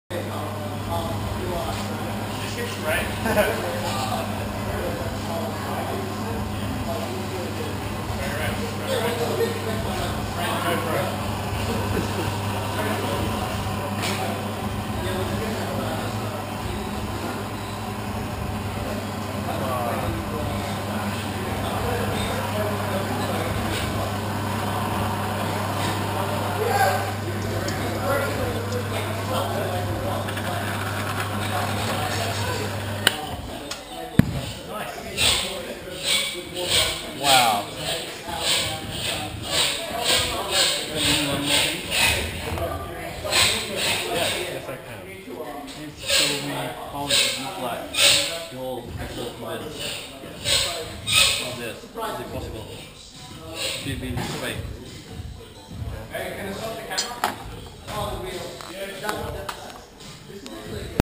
Melbourne Uni FSAE 2011: Drilling steel rod
Andrew Davies drilling mild steel rod in our first attempt to make exhaust bungs. We use a 1/8th NPT Swagelok T-piece on the dyno to get exhaust gas temps and individual cylinder lambda readings (via thermocouples and Horiba PIR-2000 CO gas analyser). These need tapped bungs to attach to the exhaust.

This rod ended up being too small to tap; cro-moly tube was tried but was also too thin wall thickness, and Davies broke the tap. Boo.

Taken with GoPro 720p/60fps. It seems like GoPro's don't like the dark.

MUR Motorsports (Melbourne University Racing)
Formula SAE-A at The University of Melbourne (Australia)